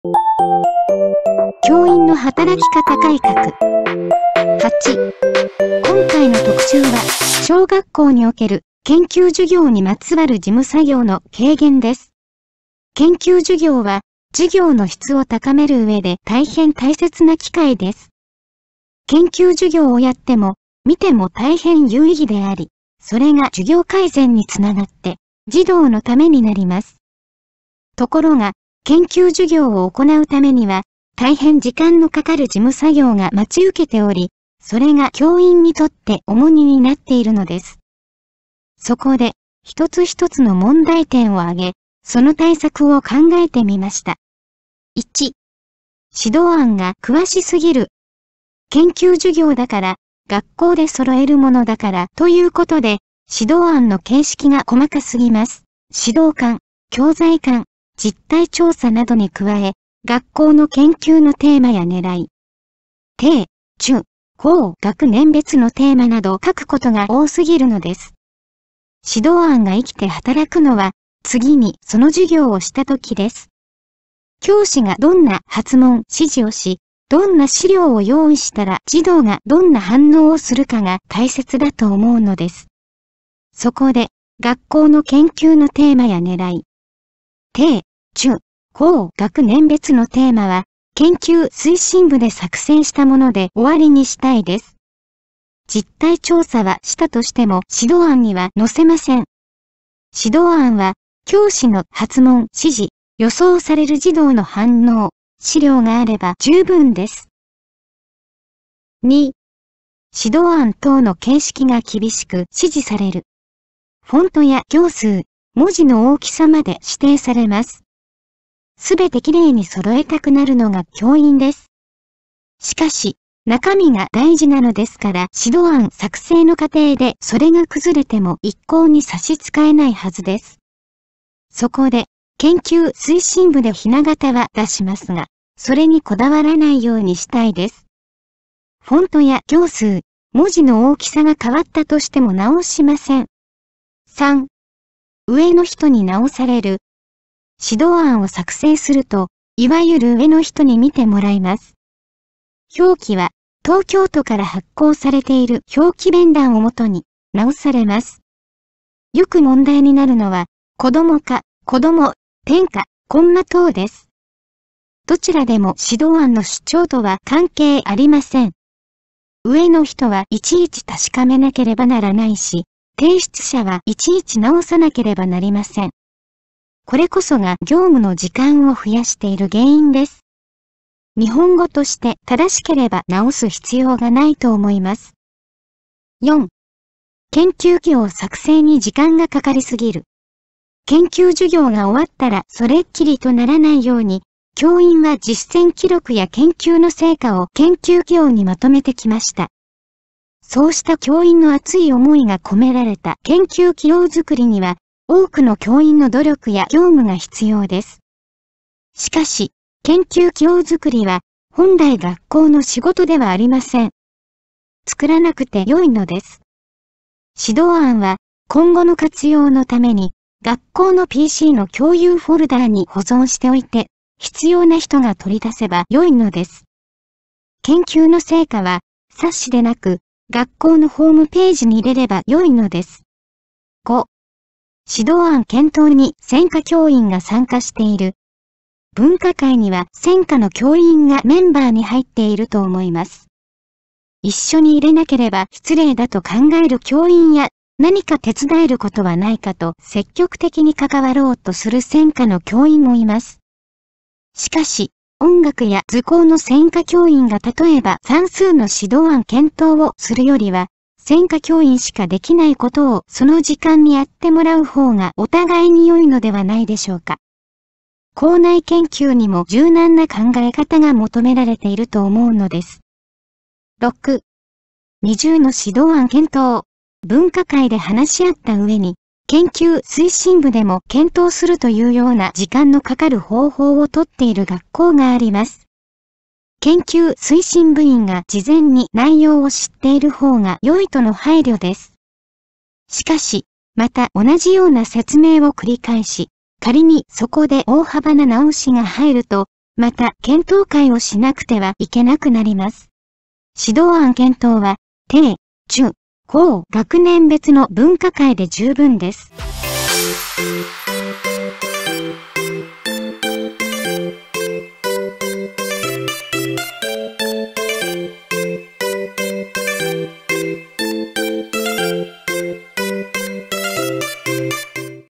研究授業にまつわる事務作業の軽減
研究授業を行うためには、大変時間のかかる事務作業が待ち受けており、それが教員にとって重荷になっているのです。そこで、１つ１つの問題点を挙げ、その対策を考えてみました。